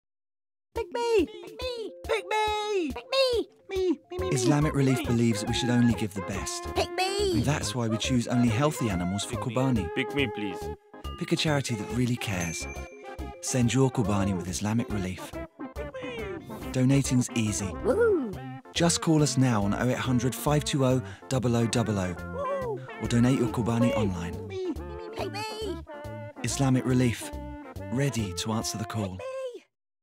Qurbani Appeal: Islamic Relief UK-Give a little love this Qurbani
With your love and support, over 2.5 million people from 27 different countries recieved fresh and quality Qurbani meat last year. This year, please help us reach many more.